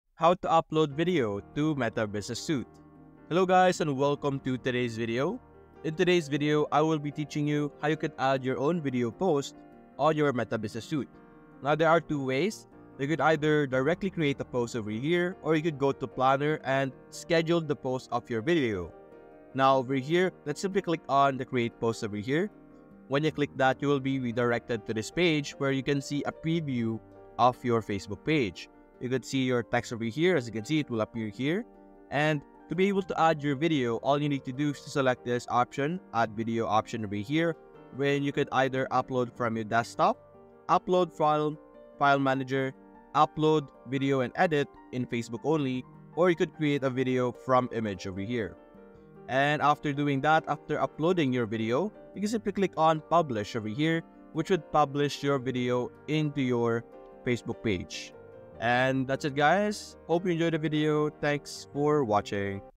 How to Upload Video to Meta Business suite (FULL GUIDE)
How to Upload Video to Meta Business suite. In this video on How to Upload Video to Meta Business suite, we are going to be teaching you how to Upload your Video to the Meta Business Suite. This video will cover everything from the basics of how to use the Meta Business Suite, to more advanced features that are available.

If you're looking to upload your video to the Meta Business Suite, then this is the video for you! I'll teach you everything you need to know to upload your video in a simple and easy-to-follow guide. After watching this video, you'll be able to upload your video to the Meta Business Suite with ease!
How to Upload Video to Meta Business suite